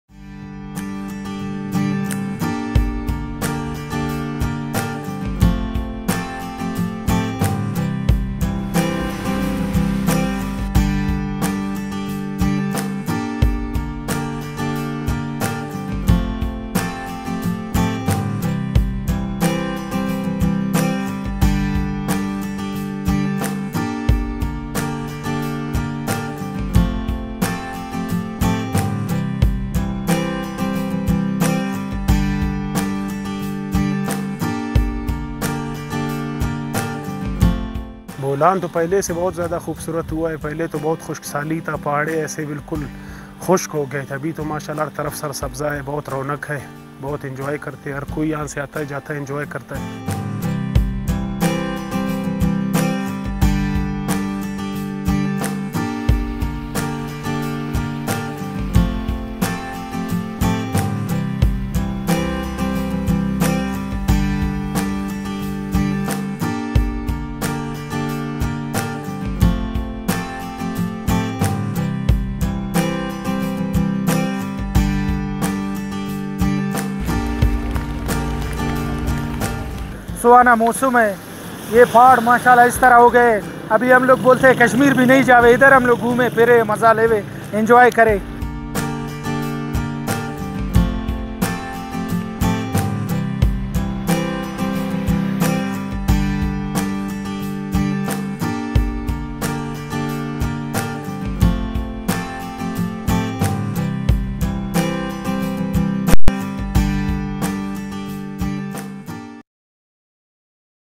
بلوچستان میں بارشوں کے بعد وادی بولان کی خوبصورتی میں اضافہ ہوگیا
بلوچستان میں بارشوں کے بعد وادی بولان کی خوبصورتی میں اضافہ ہوگیا۔ ہر سو سبزہ پھیل گیا۔ خشک اور سنگلاخ پہاڑ بھی سر سبز پہاڑوں میں تبدیل ہوگئے۔